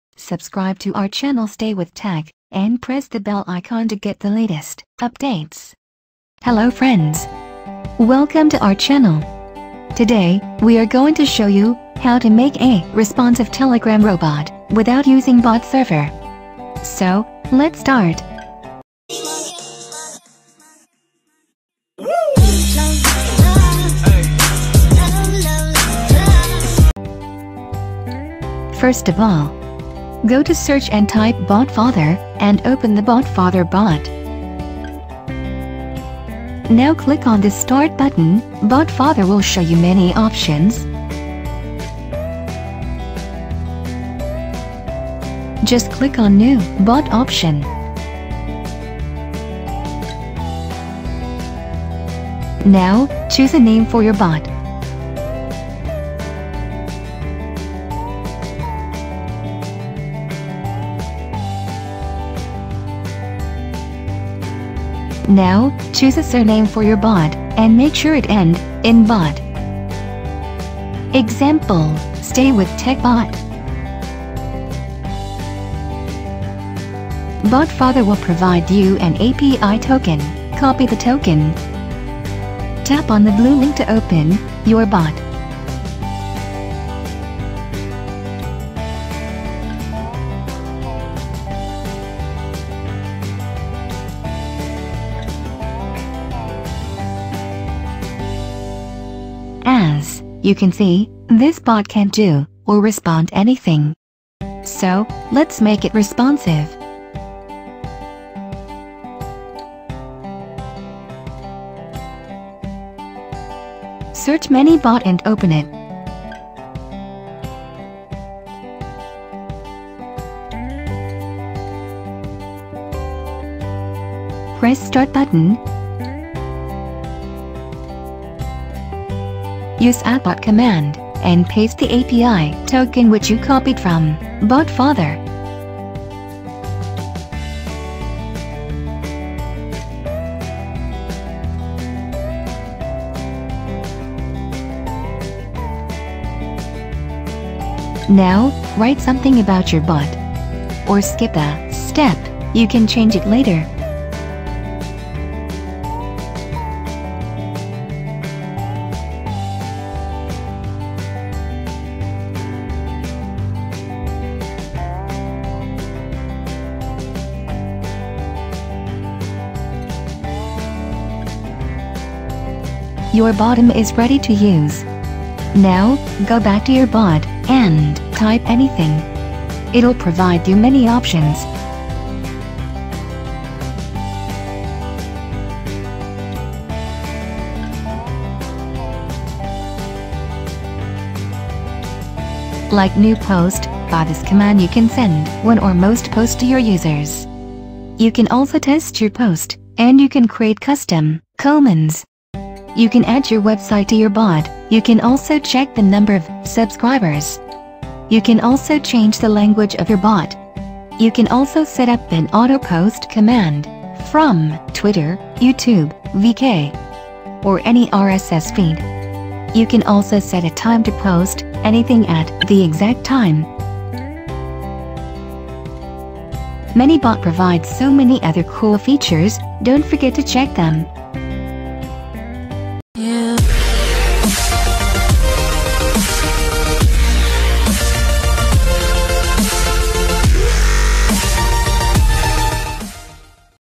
How to make a telegram auto responding robot, without server?
Hello friends, In this video we will show you, how to make a telegram bot without coding or server. You can simply create responding robot by watching this video.

In next video, we will show you the use of every commands.

Also check :-

How to successfully remove MI account of all Xiaomi devices :-

Samsung J200g (2015) FRP Bypass :-